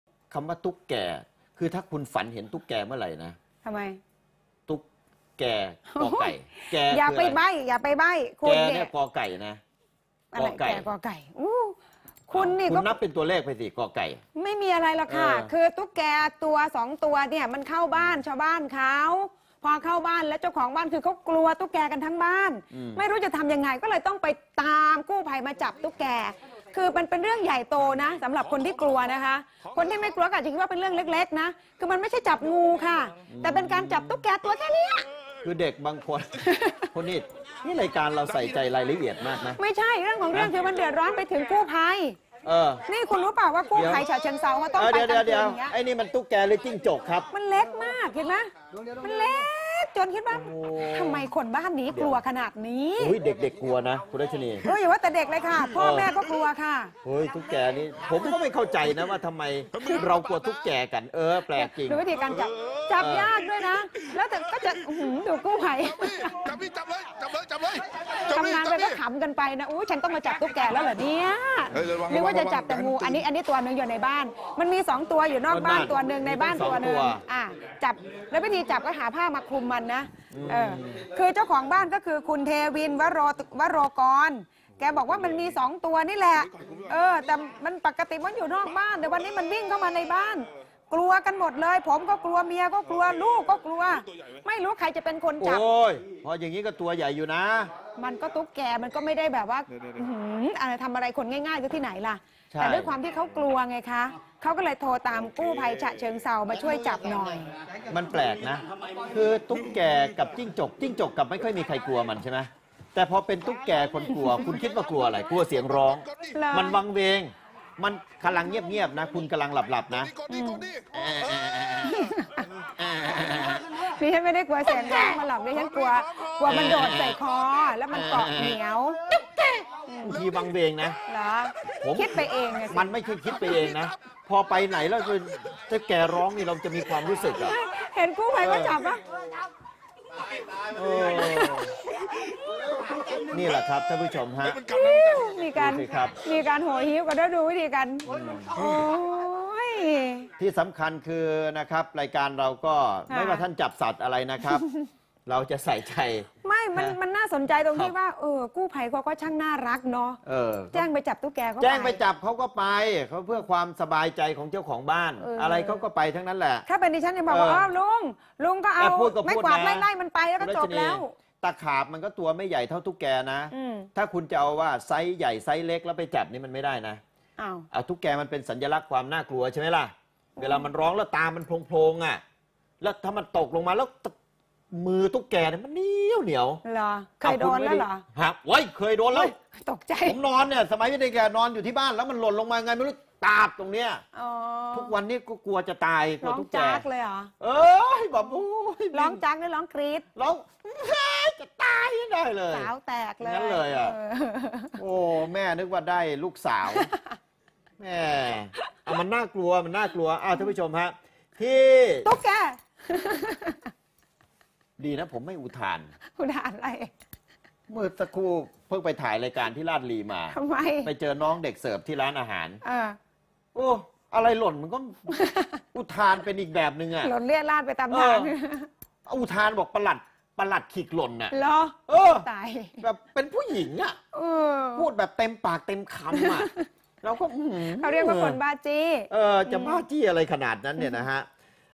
ปฏิบัติการไล่จับตุ๊กแก
เจ้าของบ้านที่จังหวัดฉะเชิงเทรา สุดกลัวตุ๊กแก 2 ตัว วิ่งไปมาทั้งในบ้านและนอกบ้าน แจ้งกู้ภัยมาช่วยจับไปที แต่ก็ใช่ว่าจะเป็นเรื่องง่าย กว่าจะจับได้

1.สารคดีเฉลิมพระเกียรติ
2.พระราชกรณียกิจ-พระมหากรุณาธิคุณ
3.คนไทยหัวใจ♥รักในหลวง